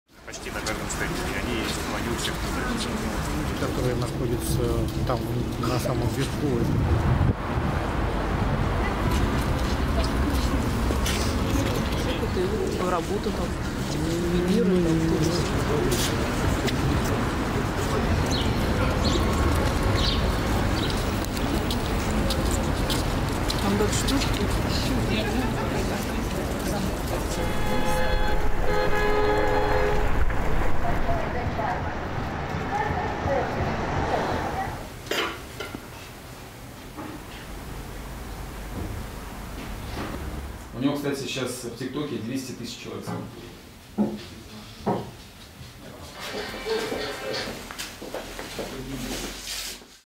Многоликий Владимир Путин
Многоликий Владимир Путин, между Че Геварой и Махатмой Ганди - продавцы сувениров рассказывают об интересе к личности российского президента, а также сезонных спадах и подъемах…